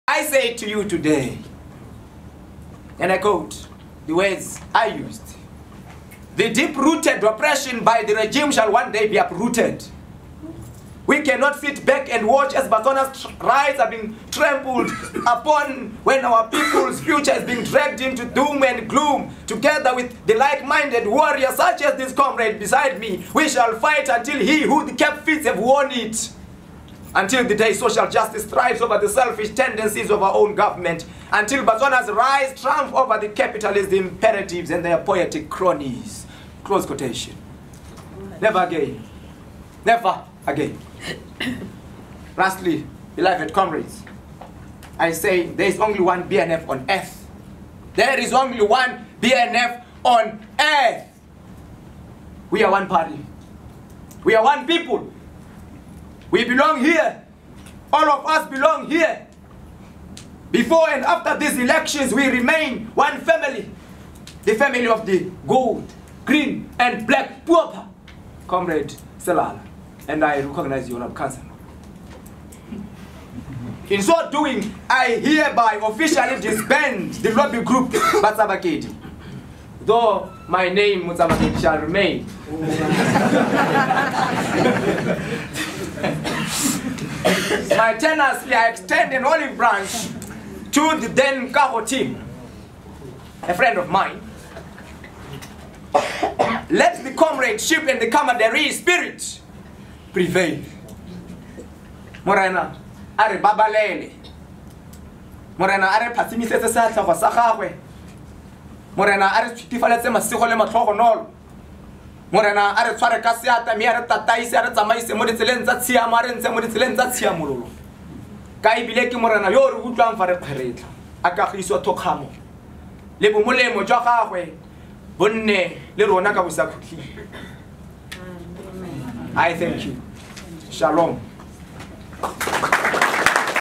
Richard 'Motsabakedi' Khumoekae - new BNFYL president
The new BNFYL president Richard 'Motsabakedi' Khumoekae at his inaugural press conference as BNFYL President in Gaborone.
Video by TSELE TSEBETSAME